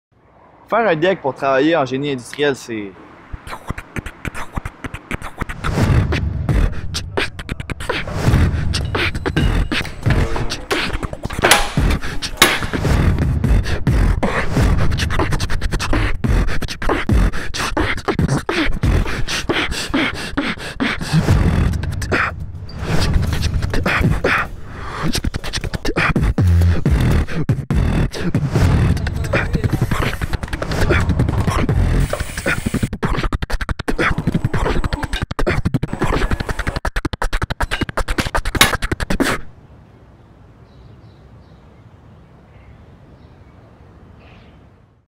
DEC en génie industriel au Collège Ahuntsic
PORTES OUVERTES LE 14 NOVEMBRE 2019 DE 14H À 20H.

Nos diplômés contribuent à enrichir l'industrie manufacturière et de services du Québec par l'optimisation des opérations et l'innovation qu'ils y apportent. C'est la raison pour laquelle les grands joueurs de l’industrie manufacturière et de services, tels que Pratt and Whitney, Bombardier, Bell Helicopter, Pharmasciences, Aéroport de Montréal, la STM et de grands hôpitaux s'arrachent nos finissants de Technologie de génie industriel   Le Collège Ahuntsic a été le premier cégep au Québec à offrir le programme Technologie du génie industriel, et est celui à l’origine de l’implantation de cette formation dans les autres collèges de la province.